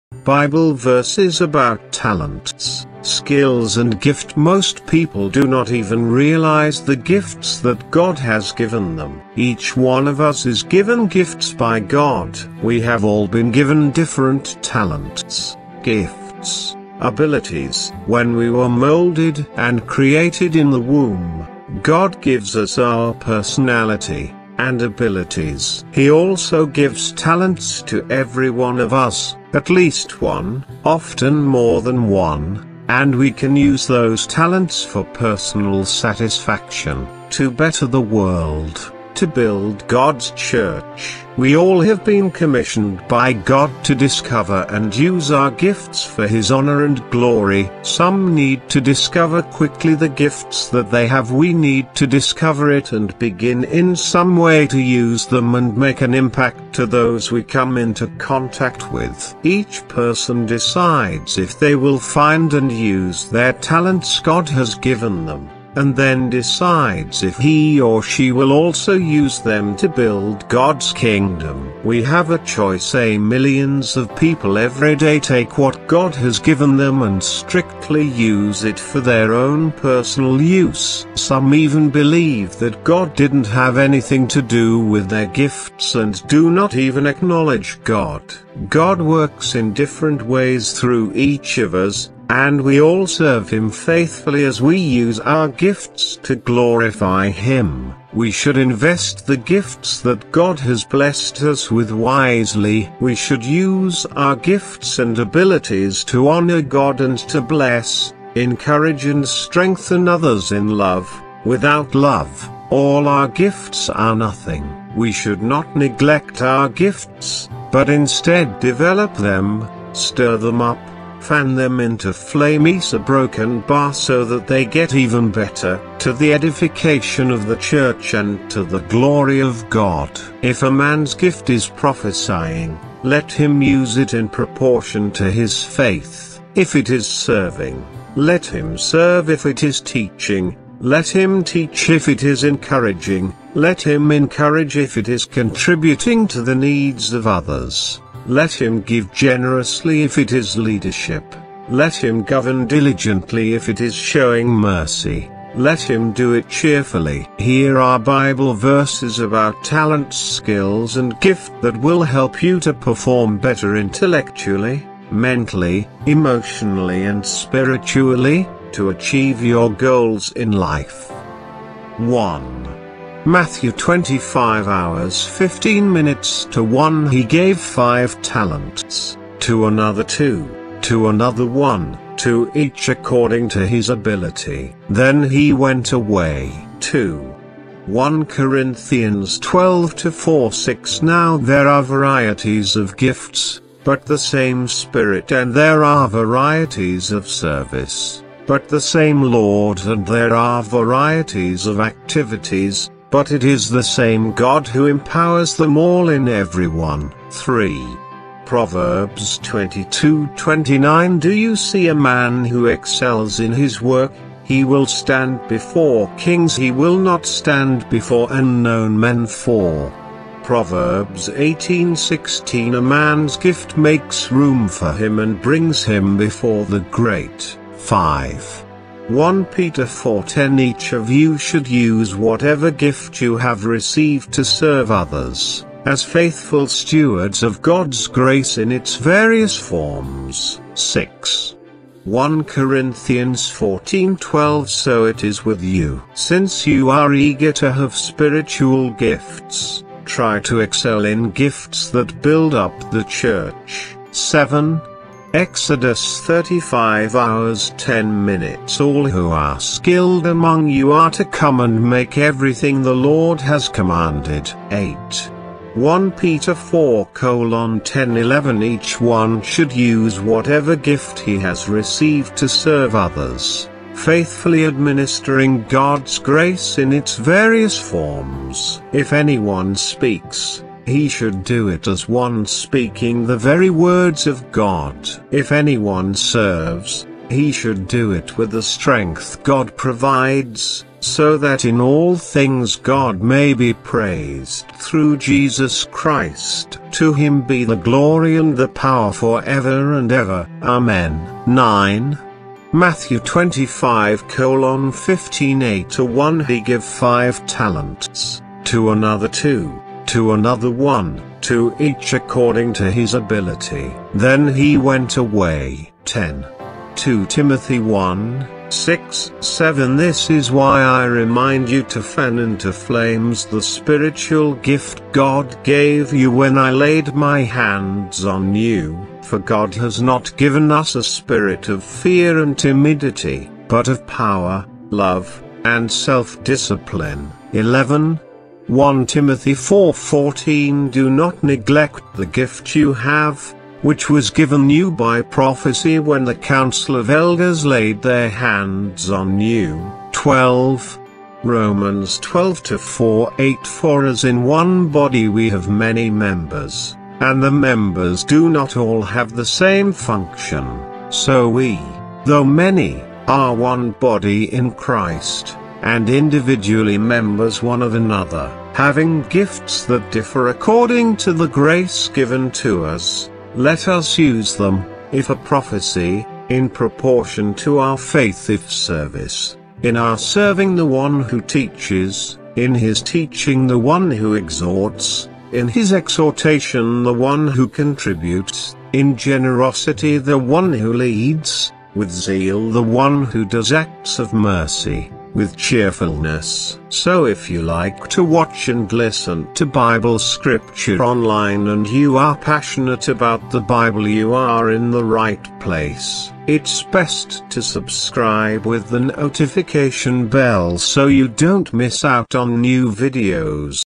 BIBLE VERSES ABOUT TALENTS, SKILLS AND GIFT
Most people do not even realize the gifts that God has given them. Each one of us is given gifts by God. We have all been given different talents, gifts, abilities. When we were molded and created in the womb, God gives us our personality, and abilities. He also gives talents to everyone of us. At least one, often more than one, and we can use those talents for personal satisfaction, to better the world, to build God’s church.
We all have been commissioned by God to discover and use our gifts for his honor and glory.
Some need to discover quickly the gifts that they have- We need to discover it and begin in some way to use them and make an impact to those we come into contact with.
Each person decides if they will find and use their talents God has given them, and then decides if he/she will also use them to build God’s kingdom.
We have a choice – millions of people everyday take what God has given them and strictly use it for their own personal use.
Some even believe that God didn’t have anything to do with their gifts and do not even acknowledge God.
 God works in different ways through each of us, and we all serve him faithfully as we use our gifts to glorify him. We should invest the gifts that God has blessed us with wisely. We should use our gifts and abilities to honor God and to bless, encourage and strengthen others in love (without love, all our gifts are nothing!).
 We should not neglect our gifts, but instead develop them, “stir them up”, “fan them into flames”… so that they get even better, to the edification of the church and to the glory of God.
“If a man’s gift is prophesying, let him use it in proportion to his faith. If it is serving, let him serve; if it is teaching, let him teach; if it is encouraging, let him encourage; if it is contributing to the needs of others, let him give generously; if it is leadership, let him govern diligently; if it is showing mercy, let him do it cheerfully.”
Here are bible verses about talents skills and gift that will help you to perform better intellectually, mentally, emotionally and spiritually, to achieve your goals in life